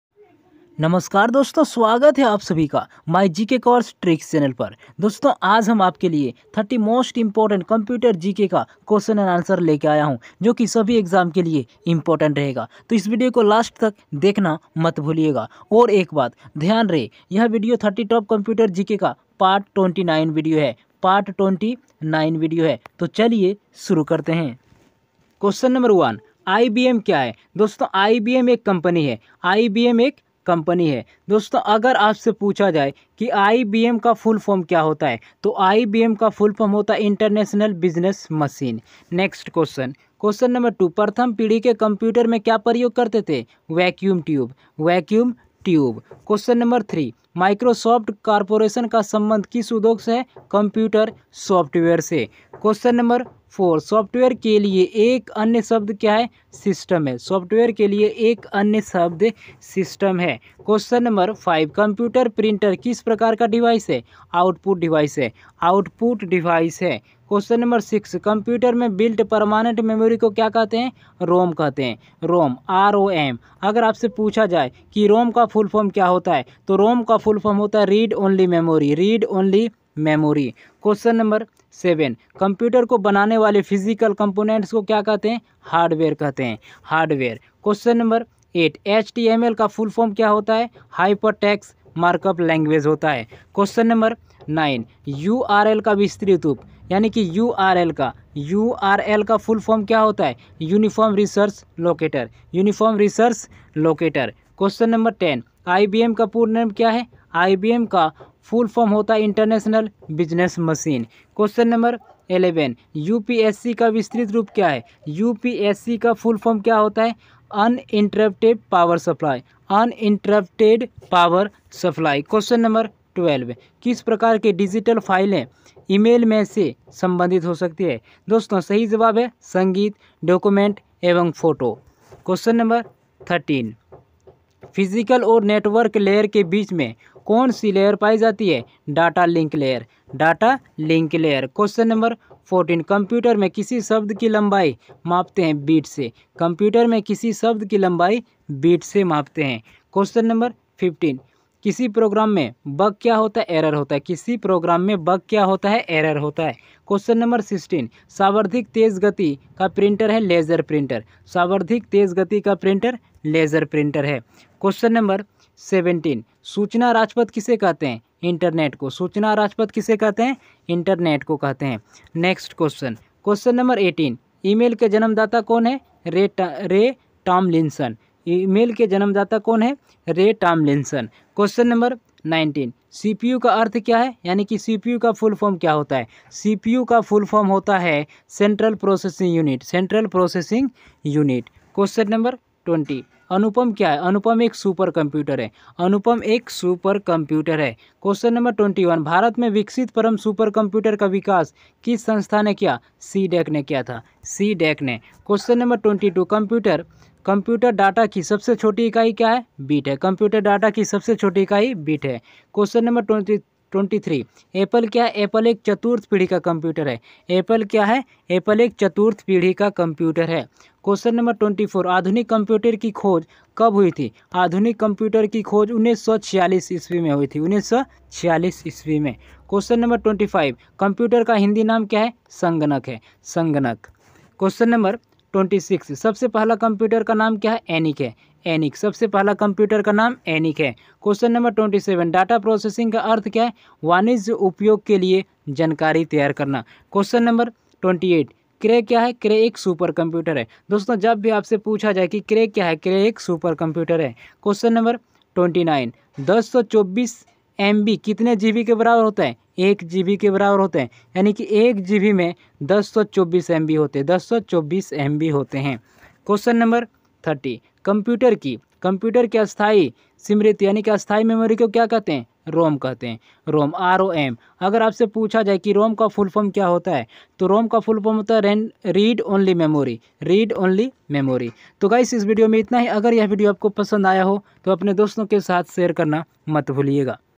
Computer| Computer basic Questions and answer in hindi |most important computer gk| lucent gk
Hello friends!
 In This video i taught 30 Most important computer gk questions and answers in hindi for all exams like- railway group d, ntpc, ssc, chsl, banking, etc.